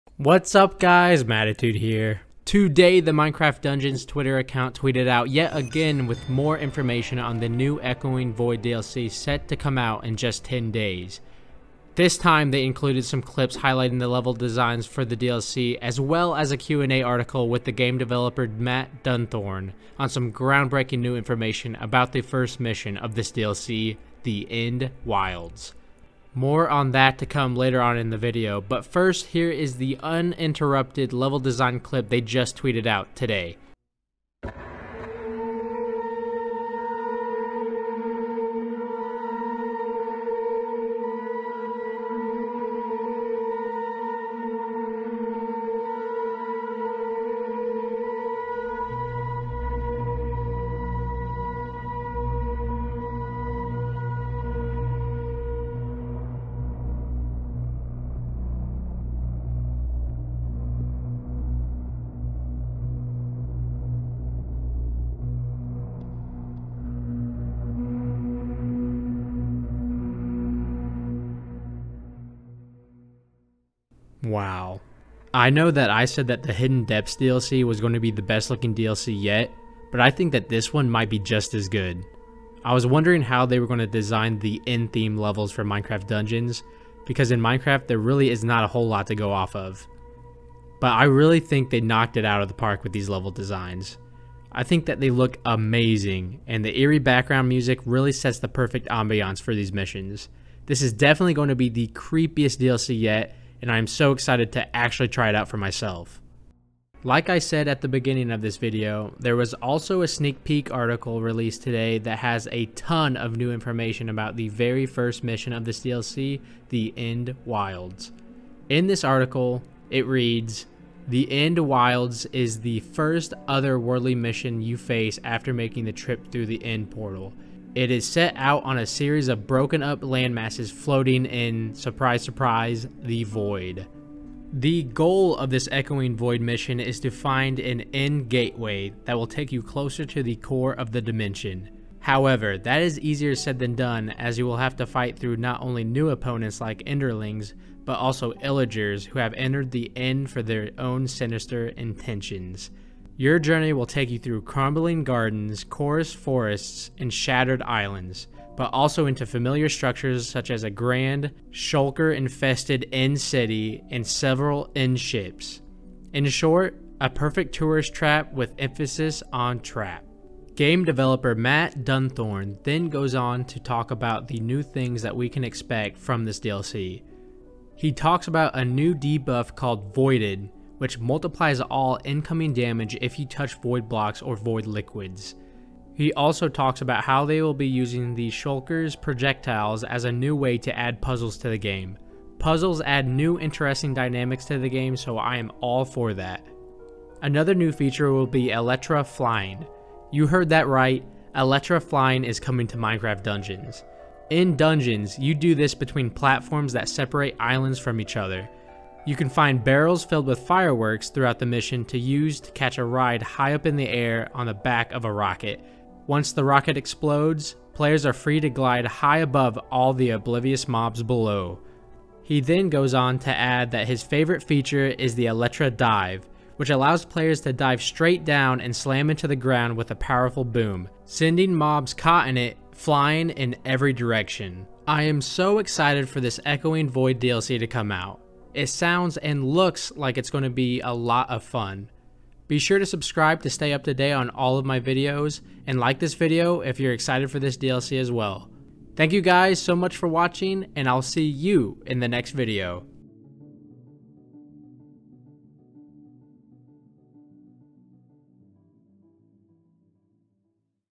FIRST LEVEL OF ECHOING VOID (The End) DLC REVEALED AND MORE - Minecraft Dungeons
In this video, I go over the big announcement today that reveals the FIRST MISSION of the Echoing Void, aka The End, Minecraft Dungeons DLC. This level is called "The End Wilds" and it is the first mission you have after entering the Ender Portal. This looks amazing!! I also go over an article that talks about all of the new stuff we can expect from the Echoing Void DLC, including ELYTRA FLYING!! You heard me right, Elytra Flying is confirmed for Minecraft Dungeons. :D